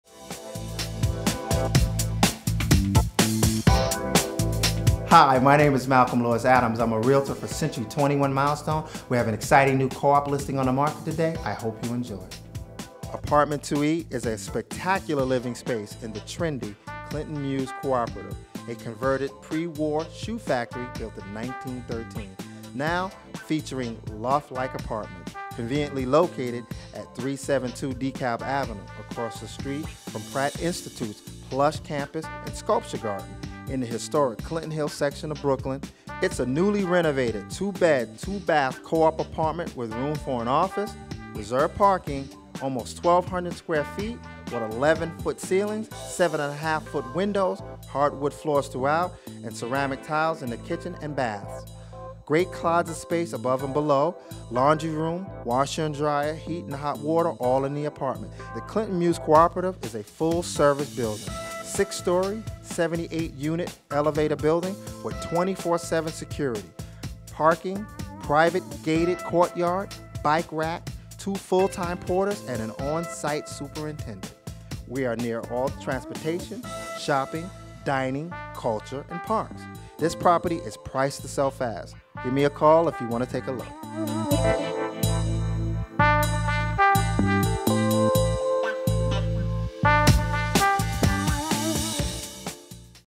The Clinton Mews Cooperative - Apt. For Sale: 372 Dekalb Ave., Unit 2E, Brooklyn NY 11205
TRENDY Clinton Mews Cooperative - Newly Renovated Kitchen, Large 2 bedroom/2 bath Coop Apartment with room for an office and a Reserved PARKING Spot (Leased Separately), 1182 Sq.ft, 11' ceilings, 7.5' windows, Hardwood Floors Through-Out, Ceramic tile in the kitchen + baths, Foyer Leads to a Spacious Living Room with Dining Area - Great Closet Space with Storage Above. Laundry Room with W/D and the Heat/Hot Water Boiler are inside the unit. PRICED TO SELL FAST!!!

Full Service, Six Story, 78 Unit Elevator Building with 24/7 Security, Parking, Private/Gated Courtyard, Bike Racks, Two Full Time Porters and On-Site Superintendent.

The Clinton Mews Cooperative is a converted Pre-War Shoe Factory built in 1913, with beautiful loft like apartments. Conveniently located at 372 DeKalb Avenue, cross the street from Pratt Institute's Plush Campus & Sculpture Park in Historic Clinton Hill Section, Brooklyn, New York. Near all transportation, shopping, dining, culture and parks.